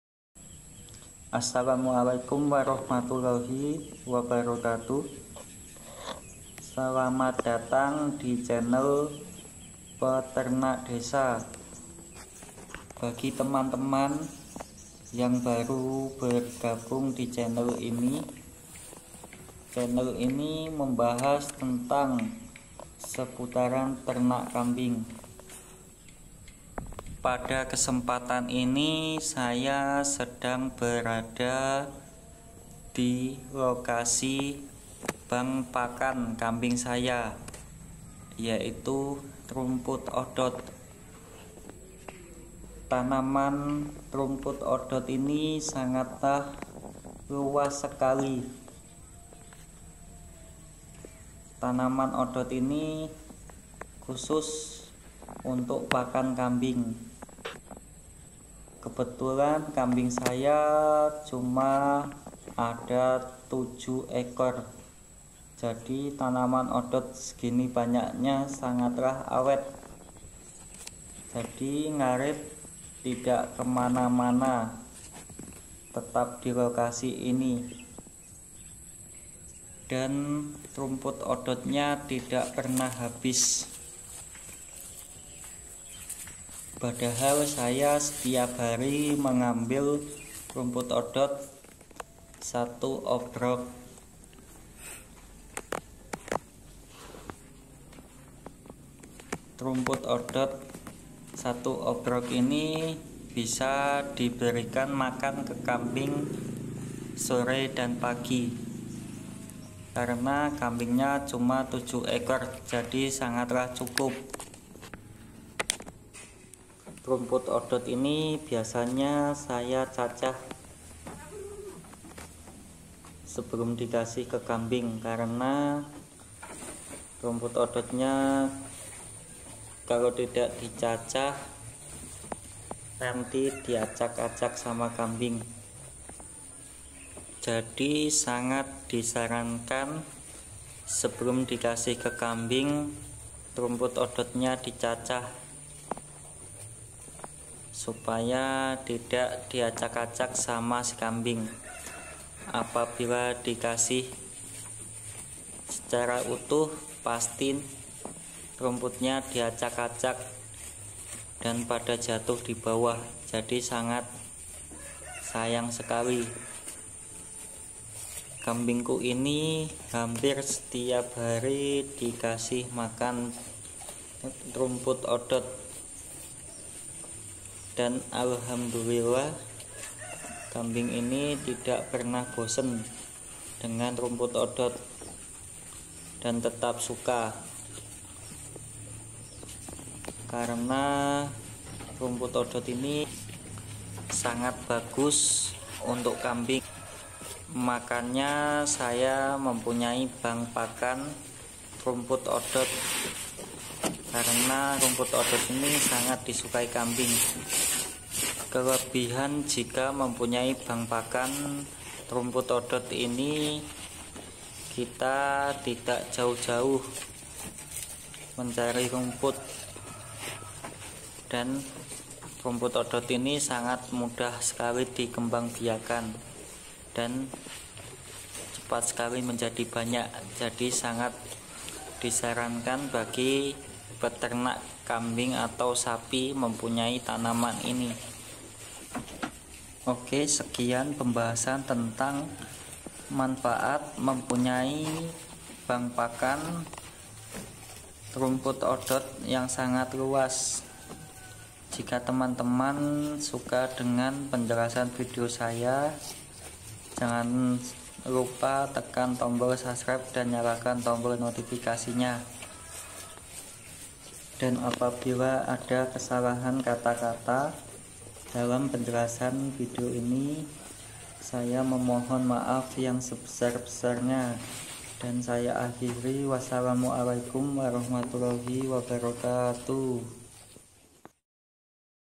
Bank Pakan Kambing Di Panen Setiap Hari Tidak Pernah Habis 💯
pentingnya Peternak Kambing Pemula mempunya Bank pakan agar pada saat ngarit tidak kemana kemana, karna sudah mempunya bank pakan rumput odot